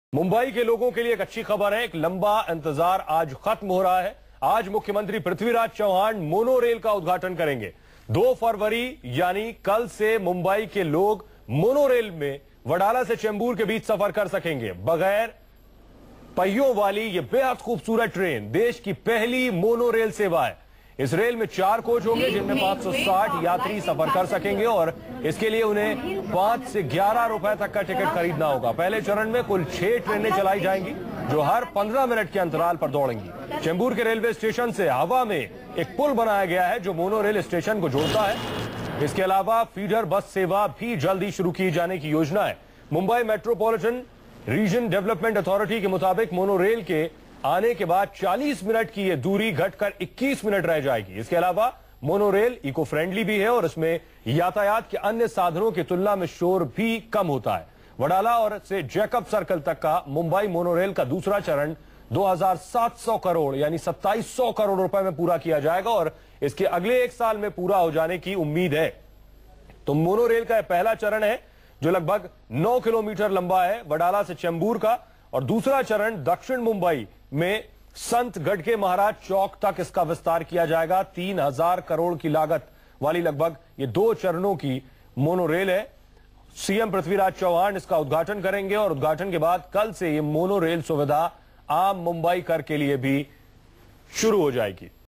India's first monorail to be inaugurated in Mumbai today
The much-awaited Mumbai monorail will be inaugurated by Maharashtra Chief Minister Prithviraj Chavan and Deputy Chief Minister Ajit Pawar today.